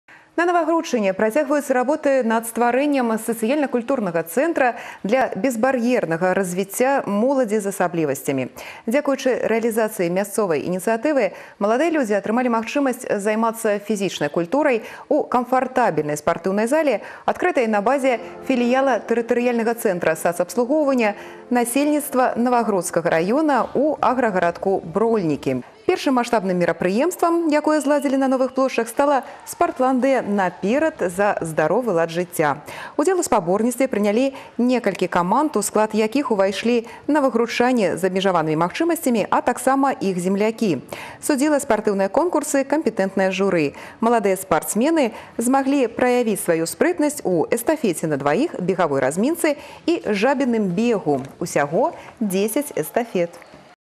Новости Гродно. Безбарьерное развитие молодежи в Новогрудке. 09.10.2017
ТРК Гродно. Новости Гродно и Гродненского региона на канале Беларусь 4 Гродно.
На Навагрудчыне працягваюцца работы над стварэннем сацыяльна-культурнага цэнтра для безбар’ернага развіцця моладзі з асаблівасцямі. Дзякуючы рэалізацыі мясцовай ініцыятывы маладыя людзі атрымалі магчымасць займацца фізічнай культурай у сучаснай камфартабельнай спартыўнай зале, адкрытай на базе філіяла тэрытарыяльнага цэнтра сацыяльнага абслугоўвання насельніцтва Навагрудскага раёна ў аграгарадку Брольнікі.